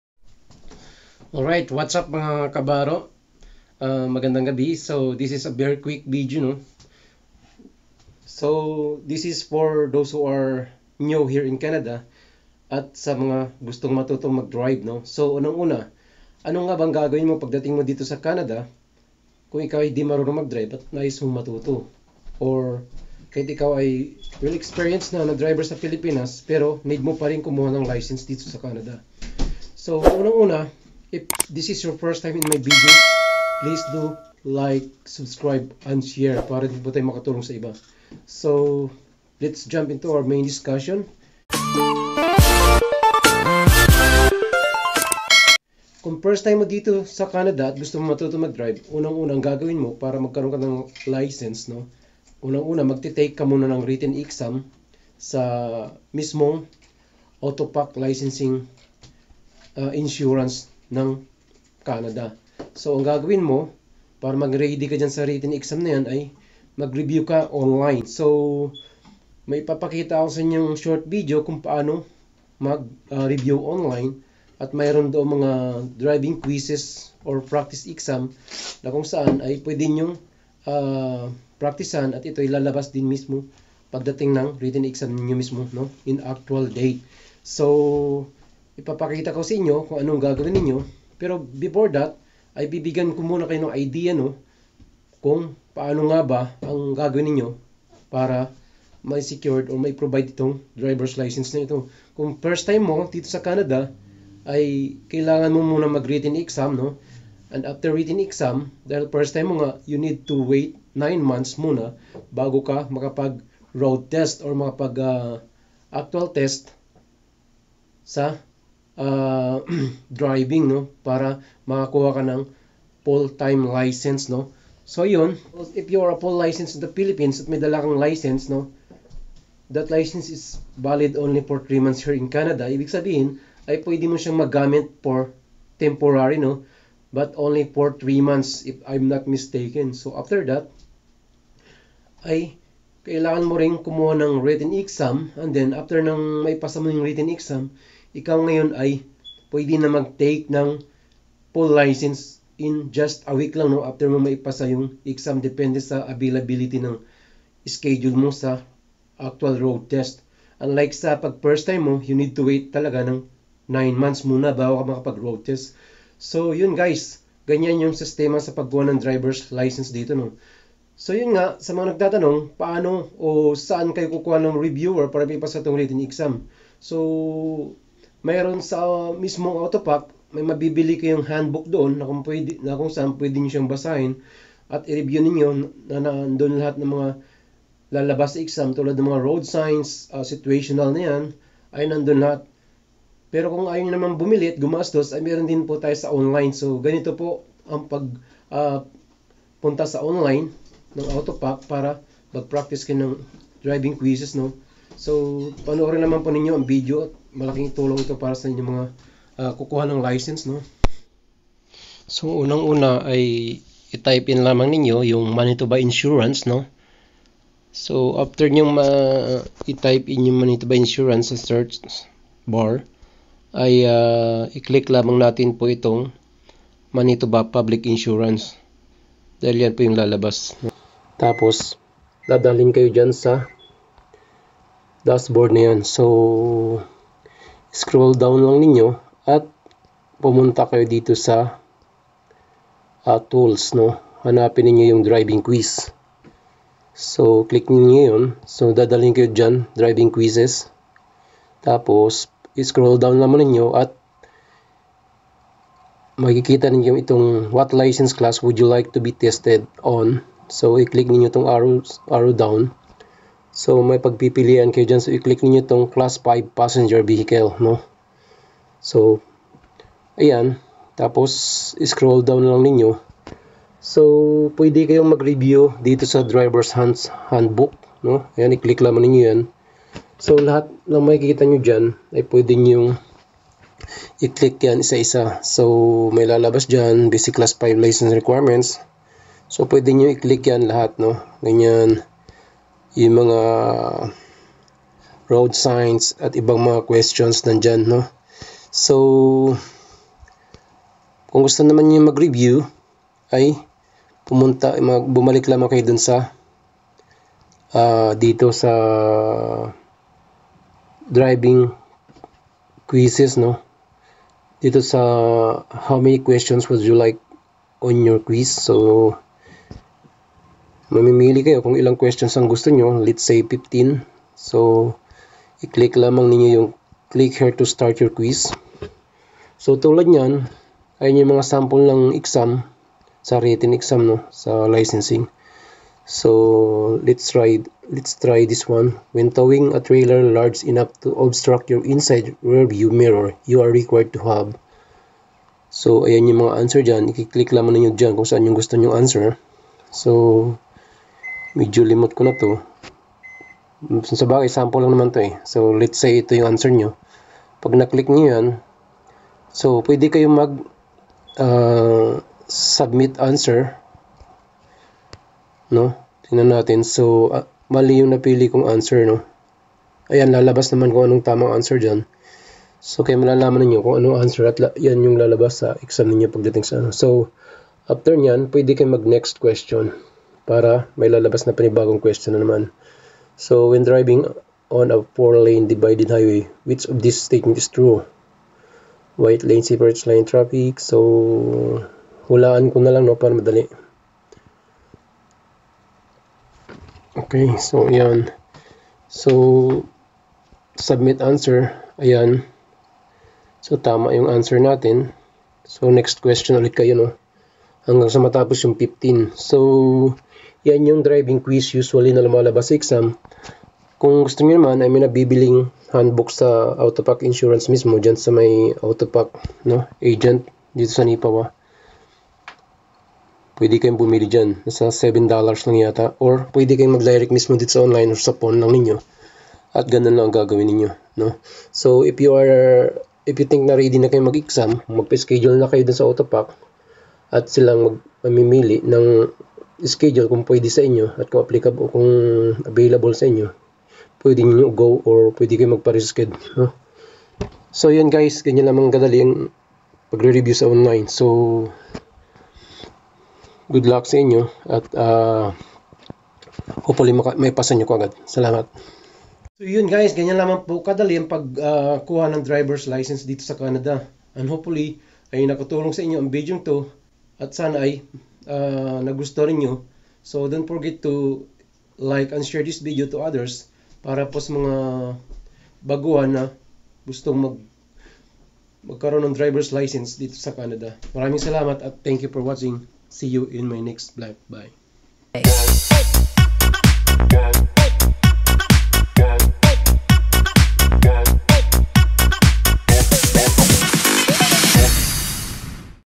HOW TO GET DRIVERS LICENSE IN MANITOBA CANADA
IF YOU ARE FIRST TIME TO GET A DRIVERS  LICENSE IN CANADA THIS MIGHT HELP YOU. WATCH UNTIL THE END